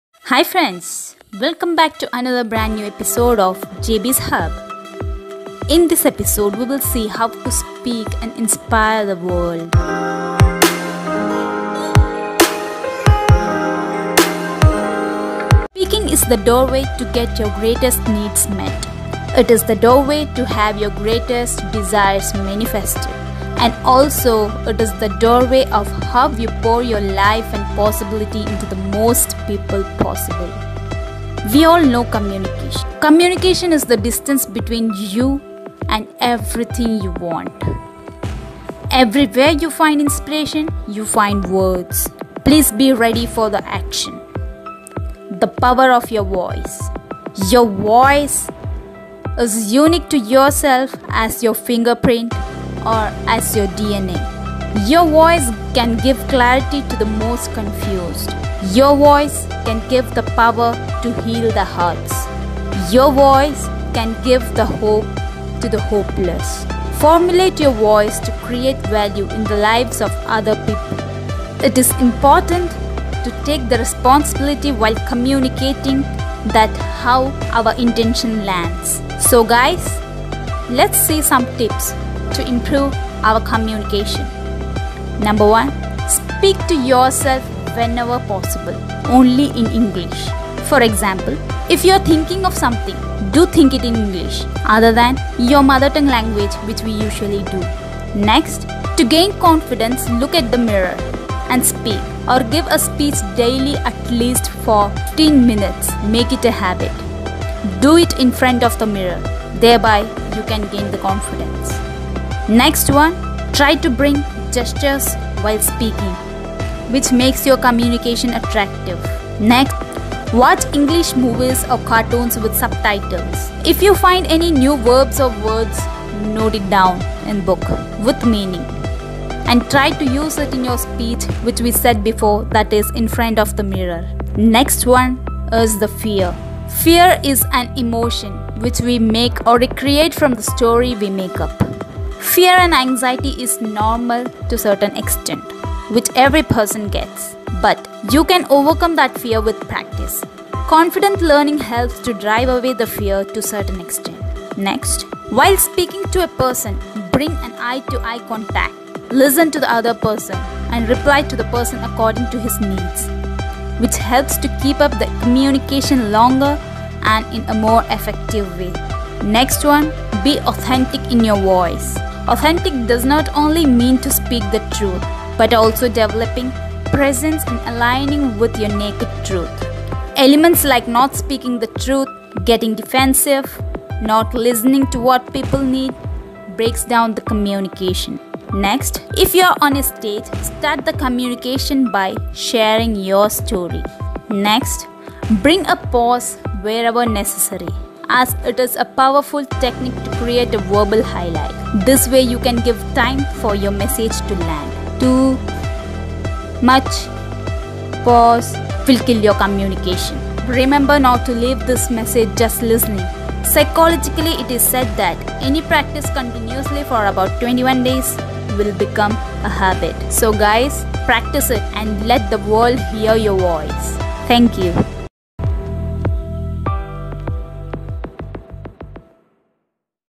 Speak & Inspire | Confident Speech | Let the World hear your Voice
Your ability to communicate is arguably the single most important skill in your life.

Simply knowing who you really are and what you really stand for allows you to speak with far greater authenticity and magnetism – and even shine as a beacon of inspiration for others.

True confidence and charisma aren’t gifts only some lucky people have – they are simply side effects of knowing how to own and project the essence of what makes you, you.

When you know how to go beyond the superficial and connect with others on a deeper level, they’ll return the favor by trusting and confiding in you.